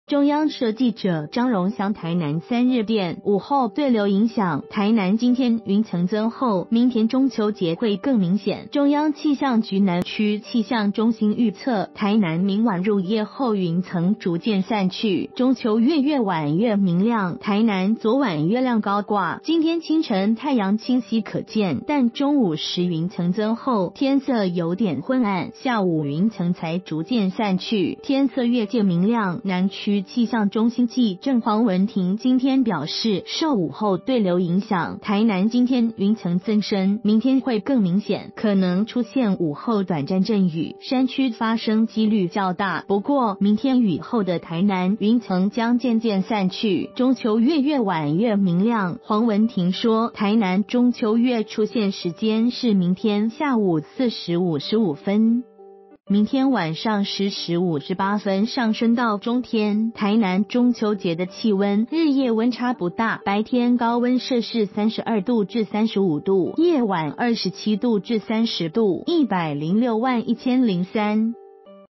午後對流影響 台南中秋月越晚越明亮
（中央社記者張榮祥台南3日電）午後對流影響，台南今天雲層增厚，明天中秋節會更明顯；中央氣象局南區氣象中心預測，台南明晚入夜後雲層逐漸散去，中秋月越晚越明亮。台南昨晚月亮高掛，今天清晨太陽清晰可見，但中午時雲層增厚，天色有點昏暗，下午雲層才逐漸散去，天色越見明亮。南區氣象中心技正黃文亭今天表示，受午後對流影響，台南今天雲層增生，明天會更明顯，可能出現午後短暫陣雨，山區發生機率較大。不過，明天雨後的台南，雲層將漸漸散去，中秋月越晚越明亮；黃文亭說，台南中秋月出現時間是明天下午4時55分，明天晚上10時58分上升到中天。台南中秋節的氣溫，日夜溫差不大，白天高溫攝氏32度至35度，夜晚27度至30度。1061003

發表評論評論如下。